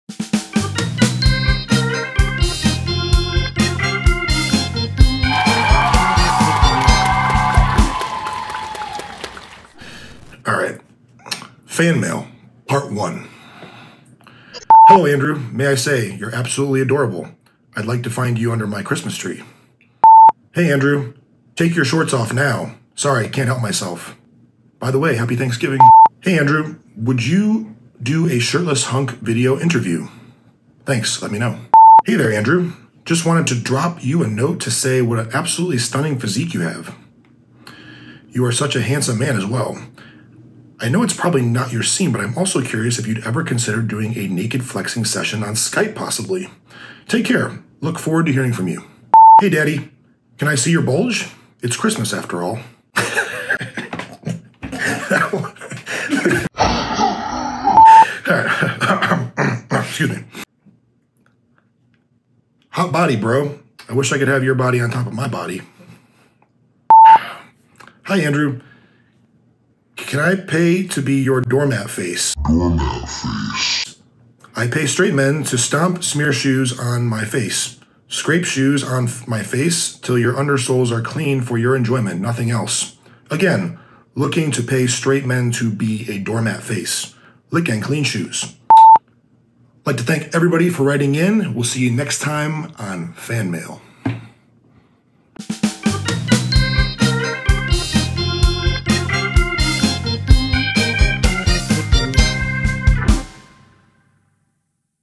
Fan Mail
In this video, I go through some of my messages from "fans".